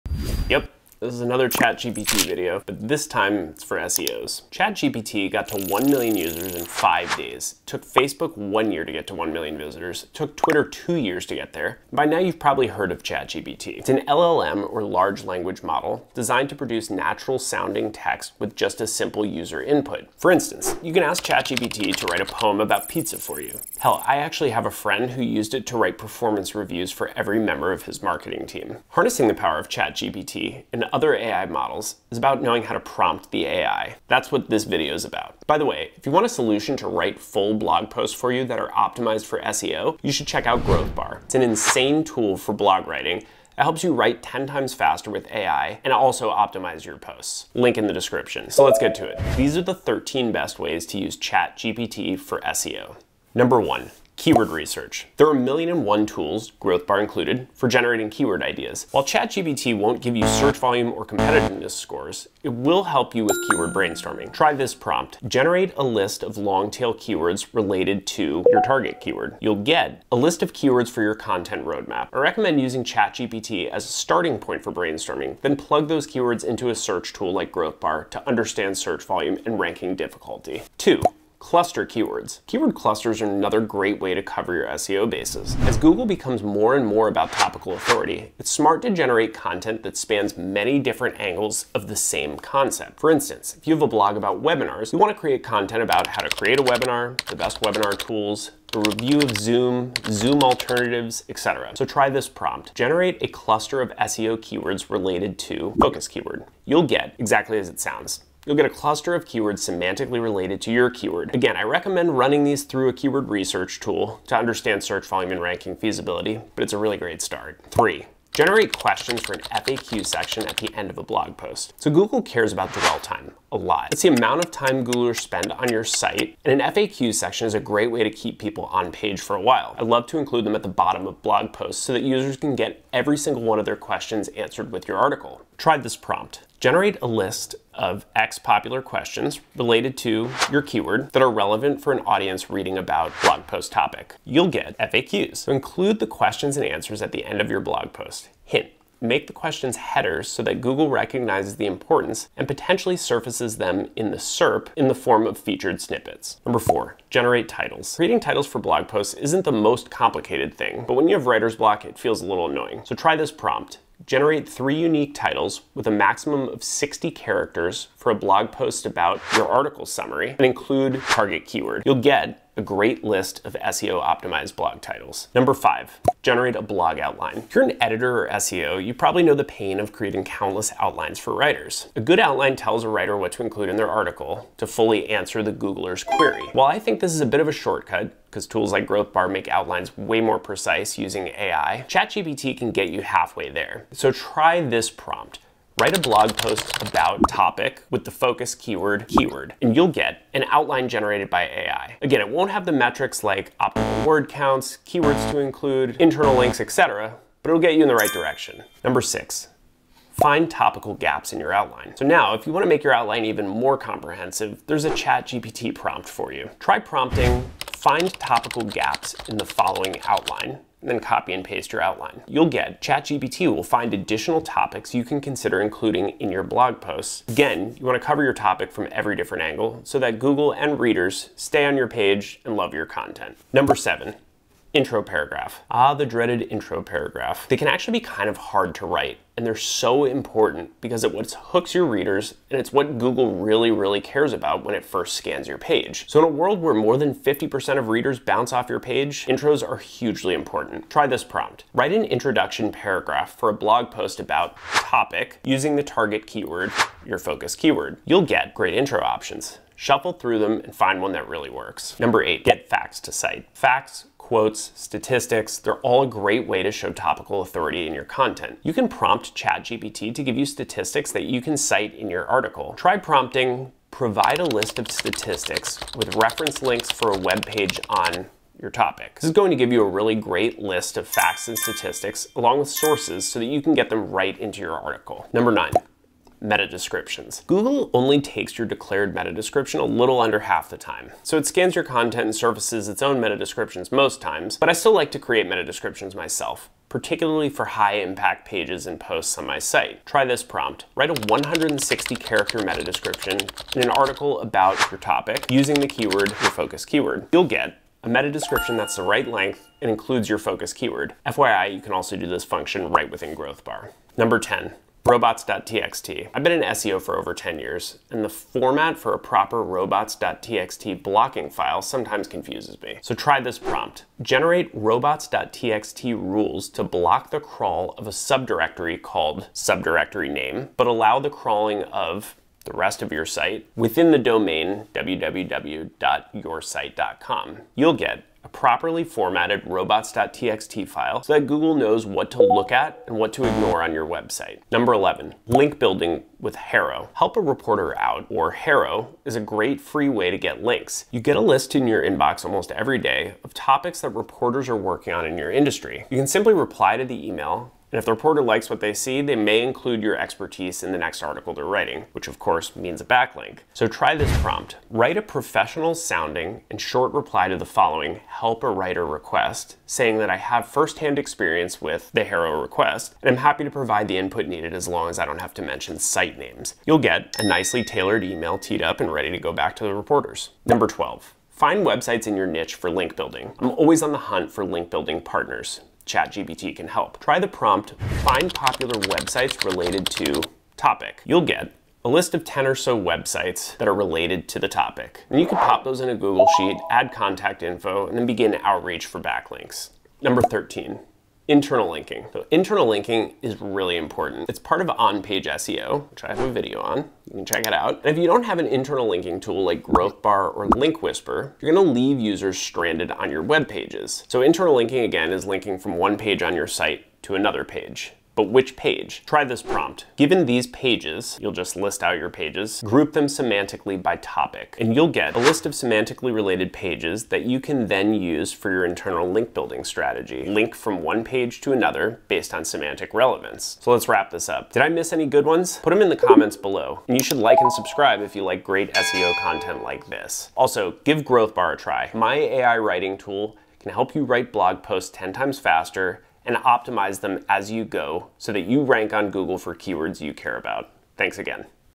SEO Hack - Chat GPT For SEO - 13 Hacks To Use Chat GPT For SEO 2023
SEO Hack - Chat GPT For SEO - 13 Hacks To Use Chat GPT For SEO 2023#seo2023

There are a ton of SEO shortcuts and growth hacks if you know how to prompt ChatGPT.

Harnessing the power of ChatGPT and other AI models is about knowing how to prompt the AI.

So let’s get into it. These are the best 13 ways to use ChatGPT for SEO:

1. Keyword research
Try: Generate a list of long-tail keywords related to [keyword].

2. Cluster keywords
Try: Generate a cluster of SEO keywords related to [keyword].

3. Generate questions for an FAQ section at the end of a blog post.
Try: Generate a list of X popular questions related to [keyword] that are relevant for an audience reading about [post topic].

4. Generate titles
Try: Generate 3 unique titles, with a maximum of 60 characters, for a blog post about [your article summary] and include [keyword].

5. Generate a blog outline
Try: Write a blog post outline about [topic] with the focus keyword [keyword].

6. Find topical gaps in your outline
Try: Find topical gaps in the following outline: [your outline].

7. Intro paragraph
Try: Write an introduction paragraph for a blog post about [topic] using the target keyword [keyword].

8. Get facts to cite
Try: Provide a list of statistics with reference links for a webpage on [topic].

9. Meta description
Try: Write a 160-character meta description for an article about [topic] using the keyword [focus keyword].

10. Robot.txt
Try: “Generate robots.txt rules to block the craw of a subdirectory called [subdirectory name] but allow the crawling of [rest of site] within the domain

11. Linkbuilding: HARO
Try: “Write a professional sounding and short reply to the following “Help a Writer” request saying that I have first-hand experience with [HARO request ] and am happy to provide the input needed as long as I don’t mention the site names: …”

12. Find websites in your niche for link building
Try: Find popular websites related to [topic].

13. Internal linking
Try: Given these [pages] group them semantically by topic.